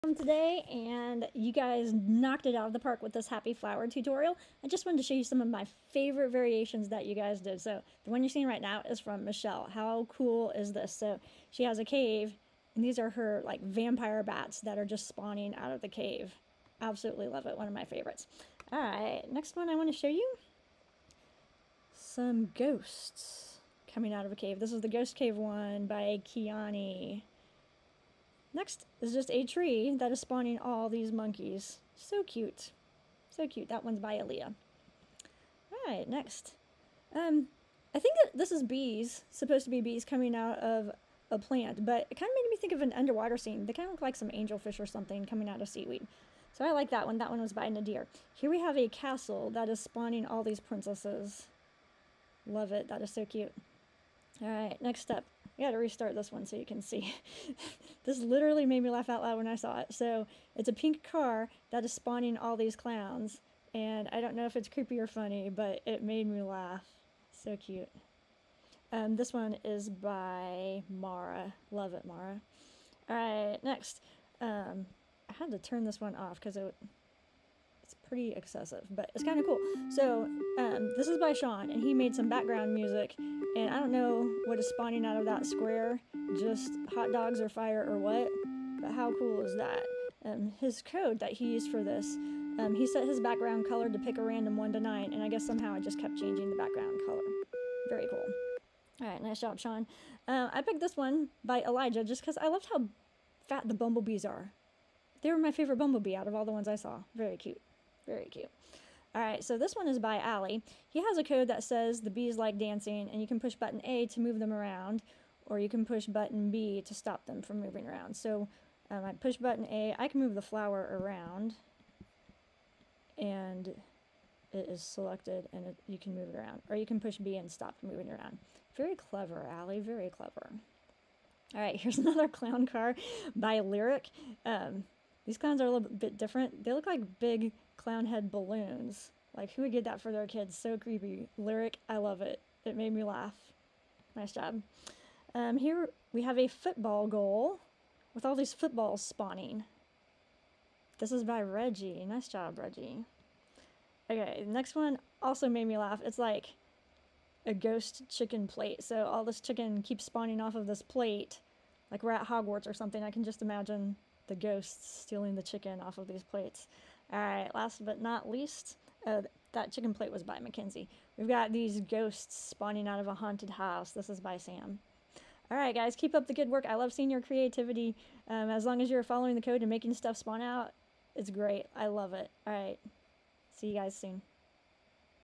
Happy Flowers Variations- just for kicks!
My students had fun following the Happy Flower tutorial and I loved the variations they came up with!  Their creativity makes my heart happy.